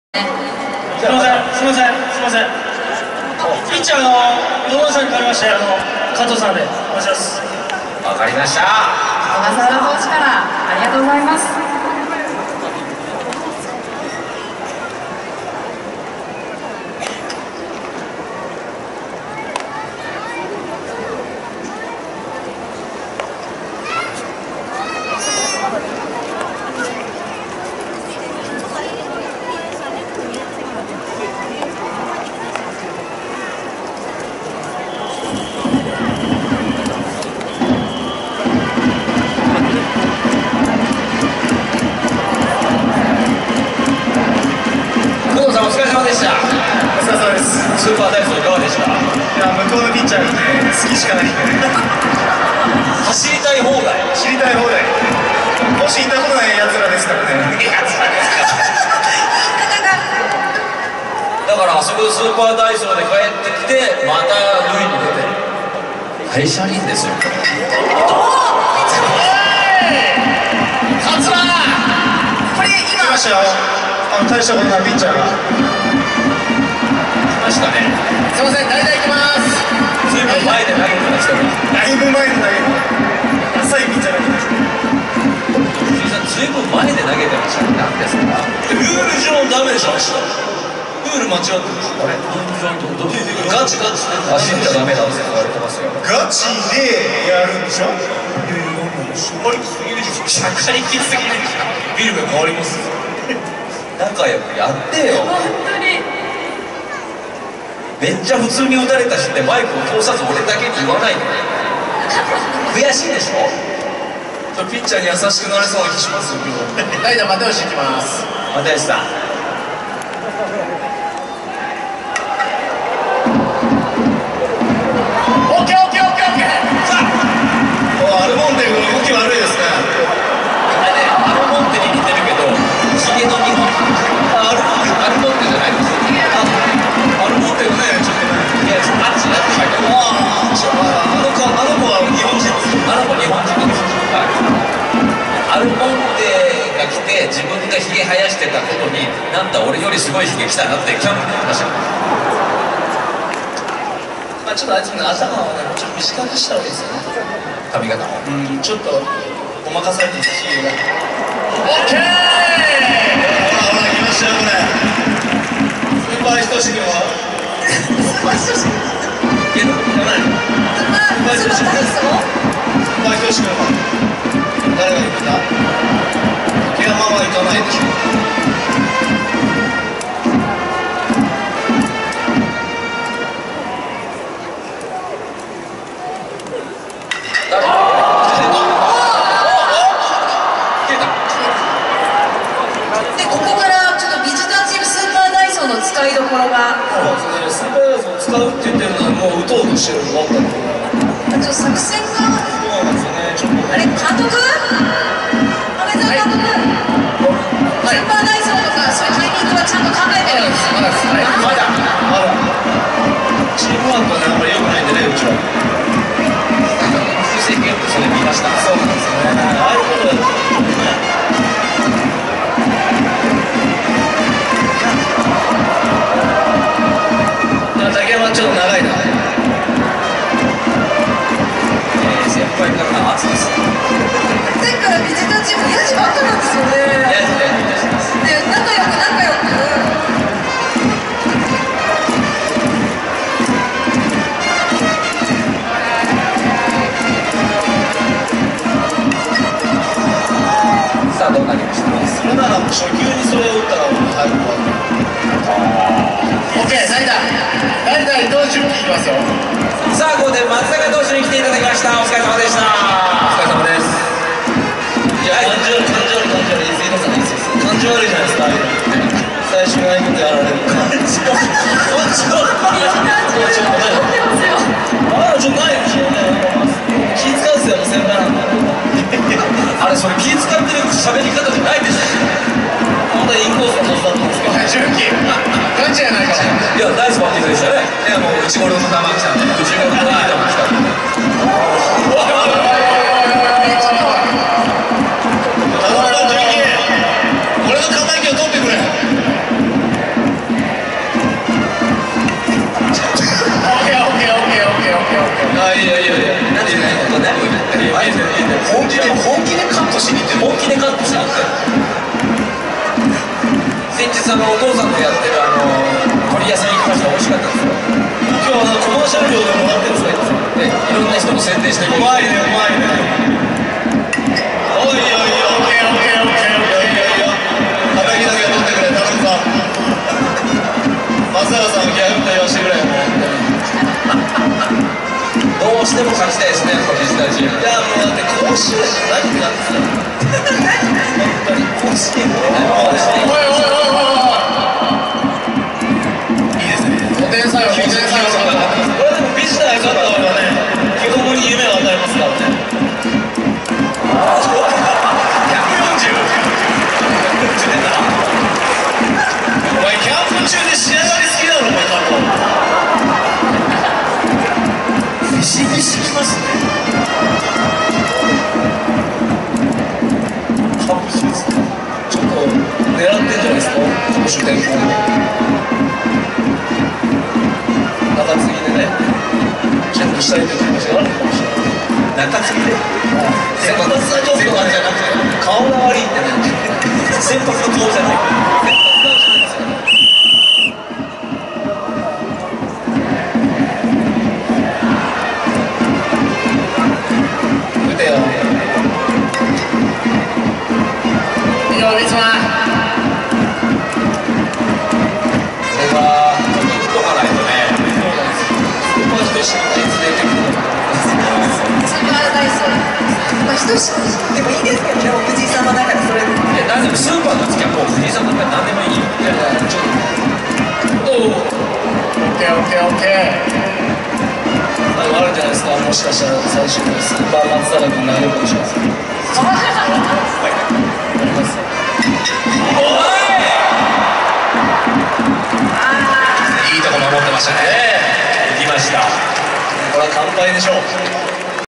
20181117 ガチンコ紅白戦、2回表！ ～ 竜キャノン加藤匠馬【ドラゴンズ ファン★フェスタ2018】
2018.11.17、中日ドラゴンズ ファン★フェスタ＠ナゴヤドーム。